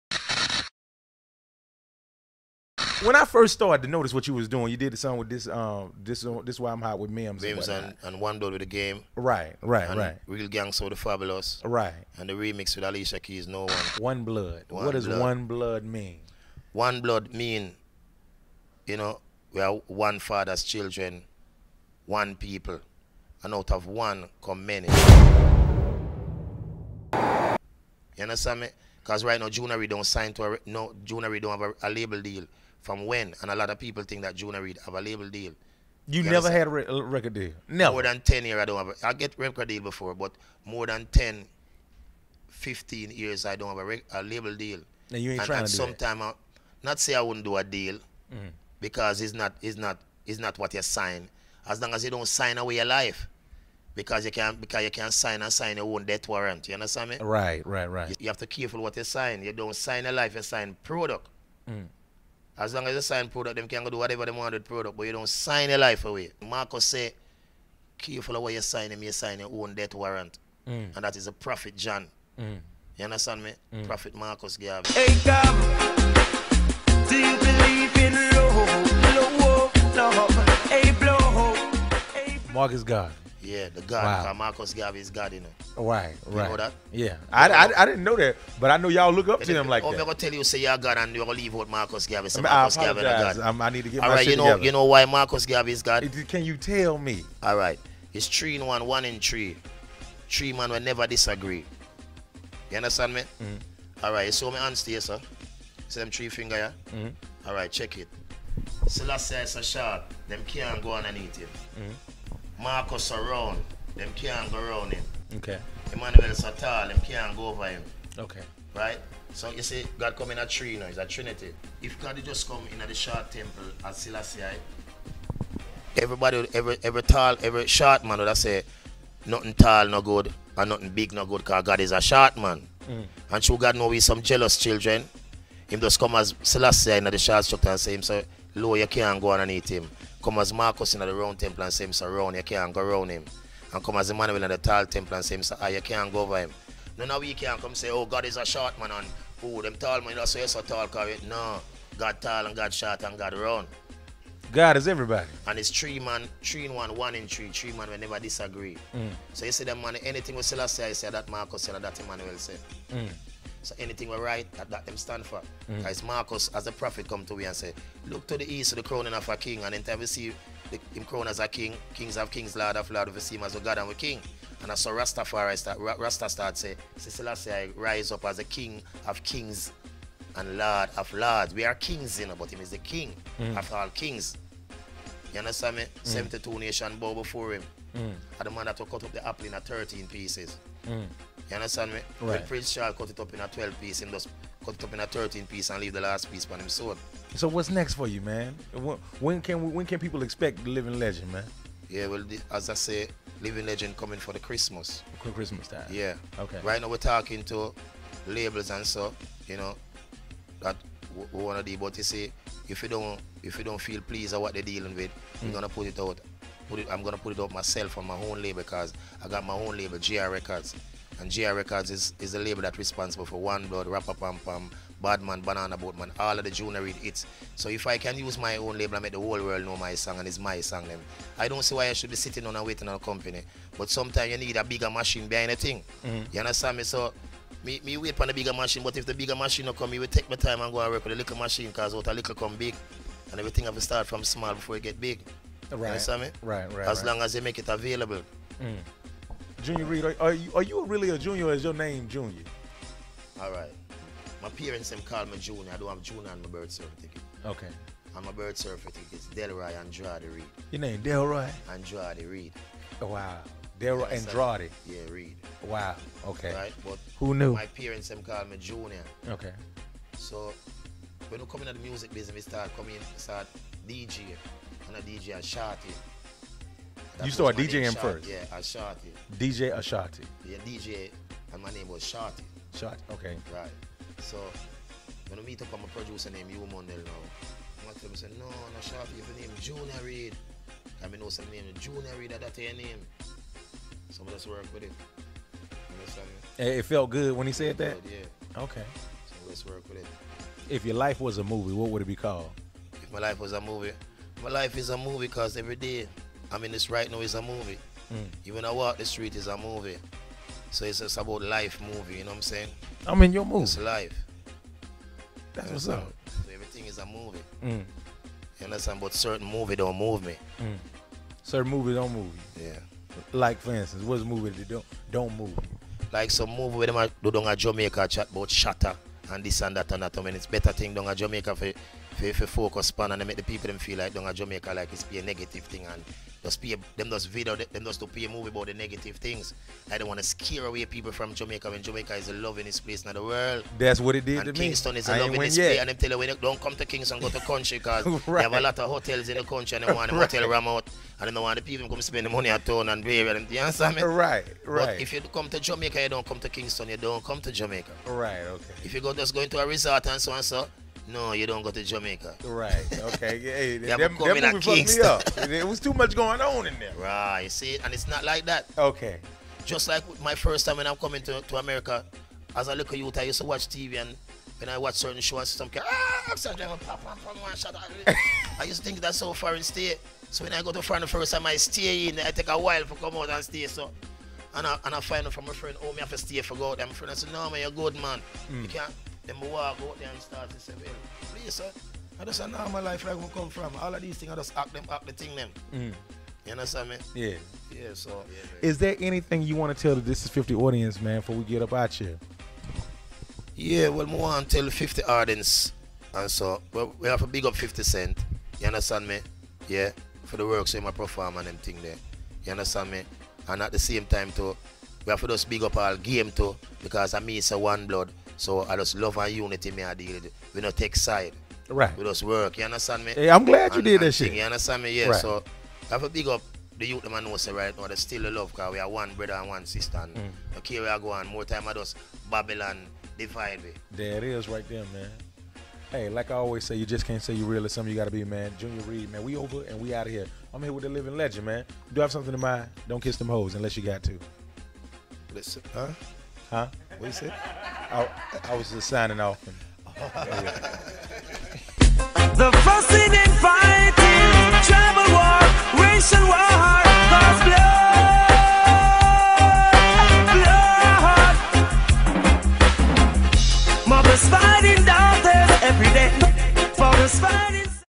Junior Reid on Marcus Garvey; His Name; Shottas Movie
Thisis50 & Young Jack Thriller recently spoke with Junior Reid for an exclusive interview!

In part 3, Junior Reid talks about the Prophet Marcus Garvey; not being on a major label, his own J R Record label and how he felt the movie Shottas made Jamaica seem dangerous & much more!

Follow @realjuniorreid @jackthriller @thisis50